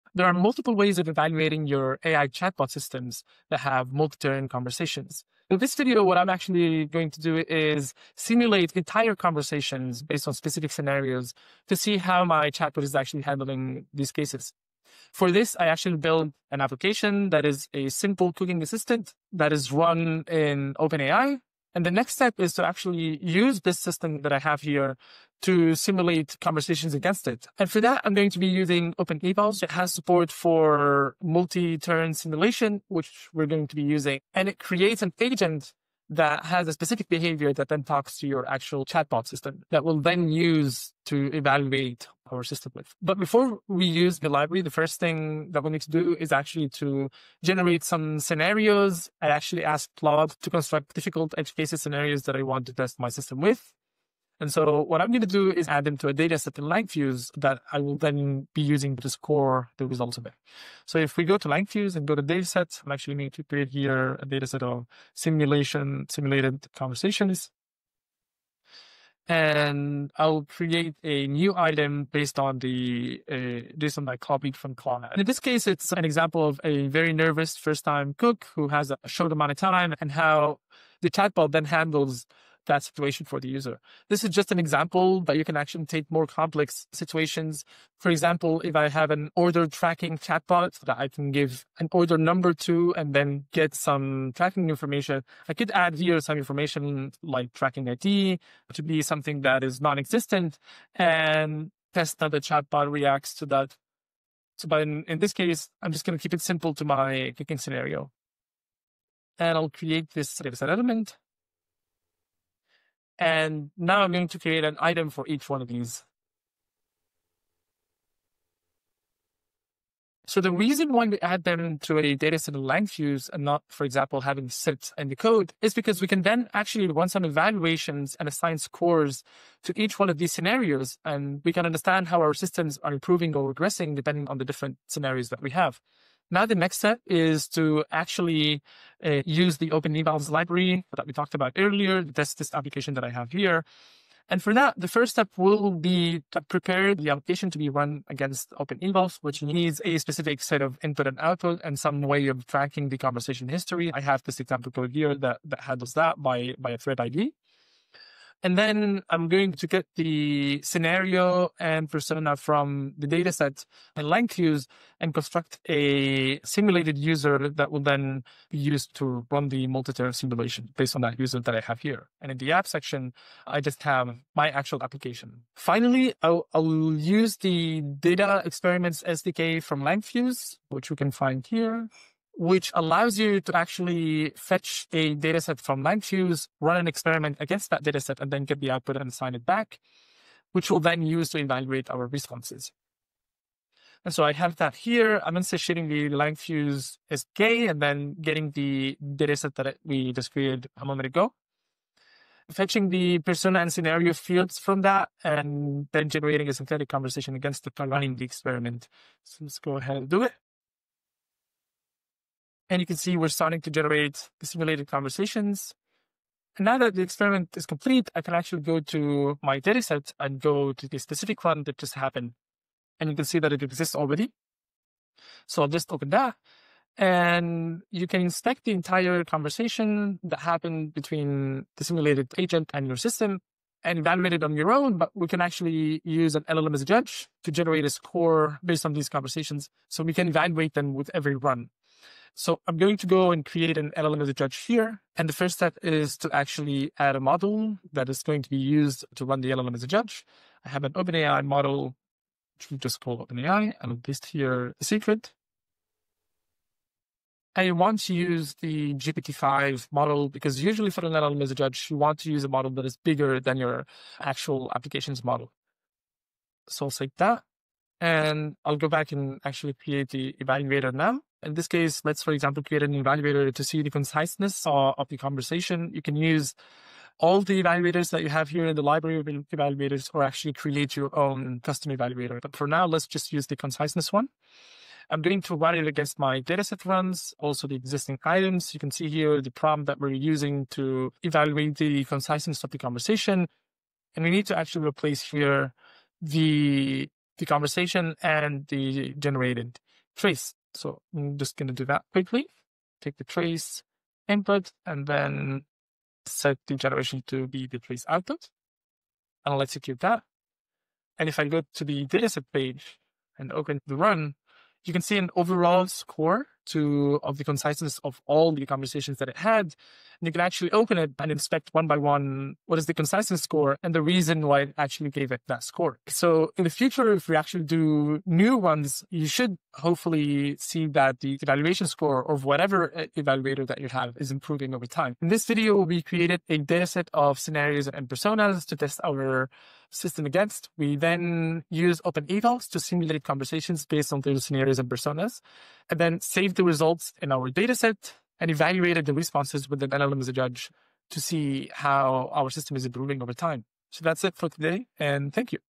Simulating and Evaluating Multi-Turn Conversations
This video demonstrates how to simulate and evaluate multi-turn conversations for AI chatbot systems. Follow along with the Jupyter notebook or with your own application traced in Langfuse.

00:00 Introduction to simulations and OpenEvals
00:43 Creating a dataset in Langfuse
02:16 Using OpenEvals with Langfuse Dataset Experiments
03:55 Using LLM-as-a-Judge to score the simulations
05:35 Summary